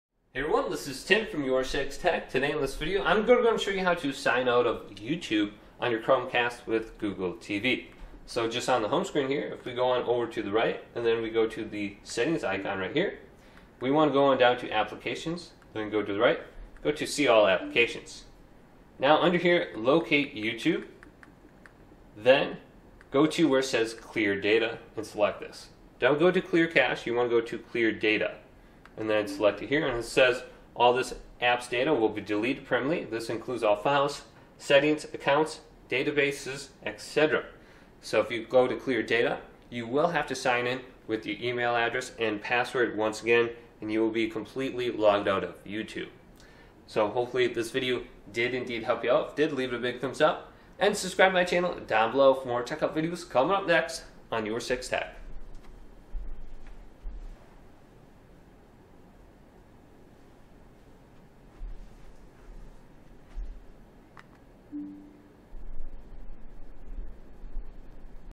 How to Sign Out of Youtube on Chromecast with Google TV (Fast Method)
Do you want to know how to sign out of youtube on chromecast with google tv! To do this, go to settings, then go to apps, then go to clear data and you will be signed out of your account. Then you will need to sign back in to youtube.